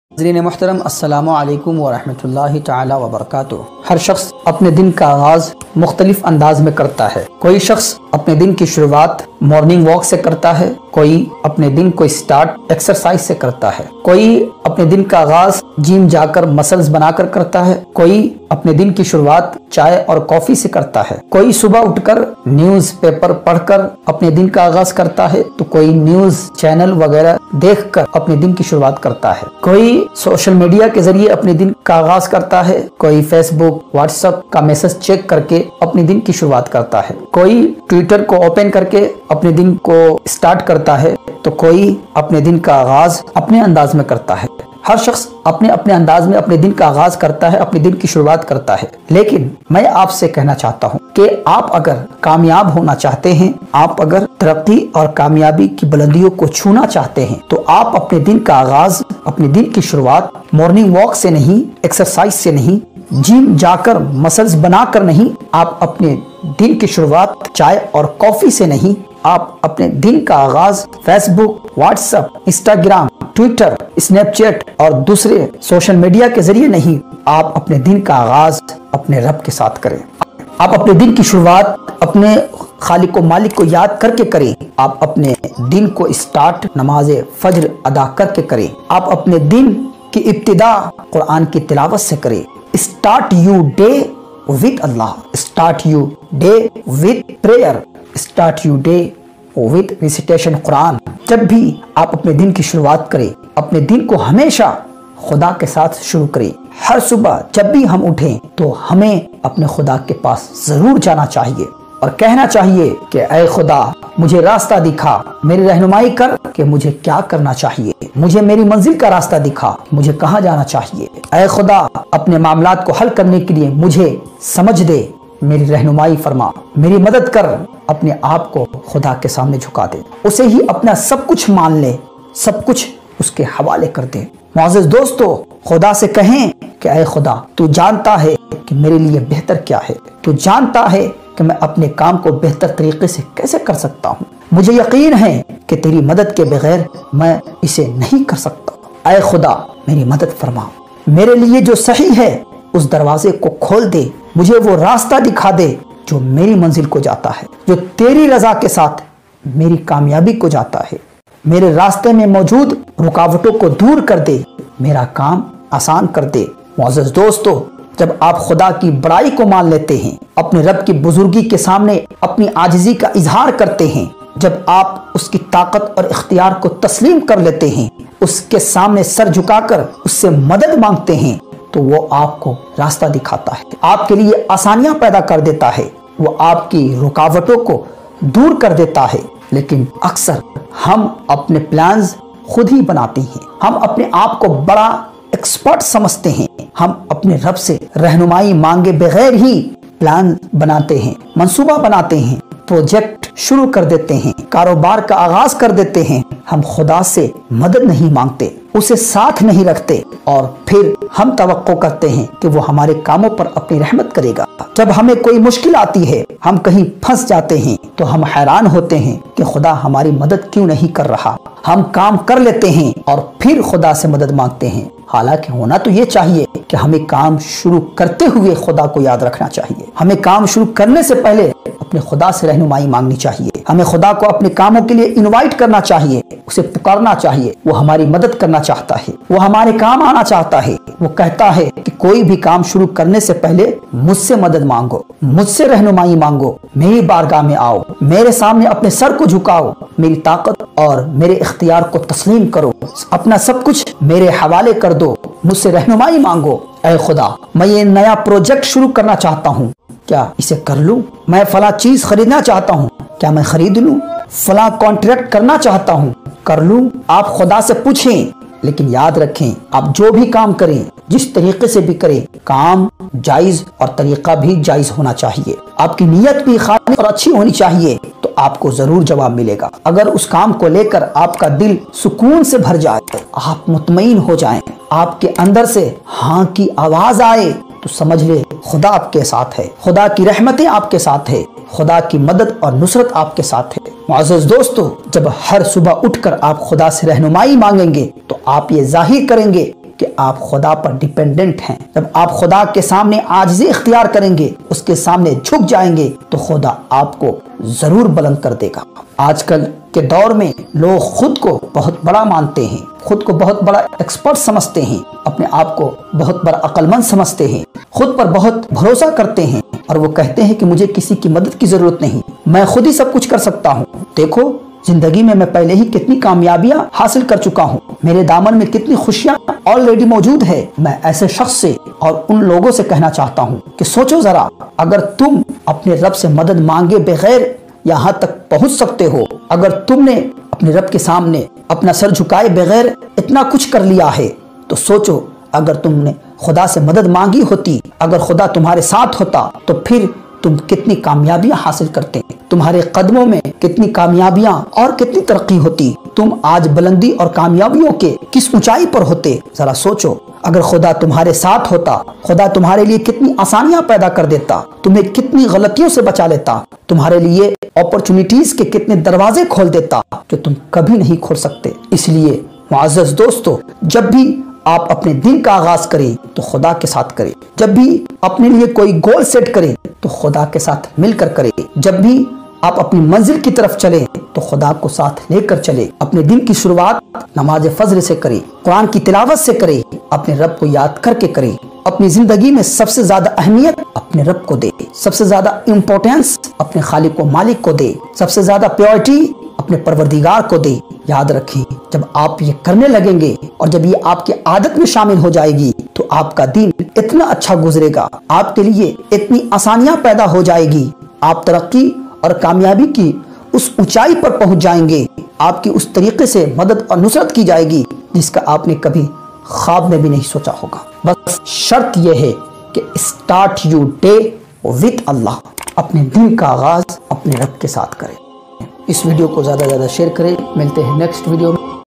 आप ‌अपने दिन की शुरुआत अपने रब के साथ करो। Start Your Day With Allah#success#life#2022
In this video We will discuss About importance Of Almighty Allah.
We will discuss About importance of Fajar Prayer.
We will Talking about importance of Recitation Quran.
In this video We will discuss About that how to Start Your Day?
How to Start Your Successful day?
how to Start Your Successful Life in 2022?
How To Change your personality in 2022?
How to Improve your Positive habits?
How to Achieve Your Goals?
How To make Your Life Happy?
How to face problems in your life?
Start your day with Almighty Allah.
Start your day with recitation Quran...
Start your day with Fajar Prayer.
apart these things many points we will discuss in this video. Inshaallah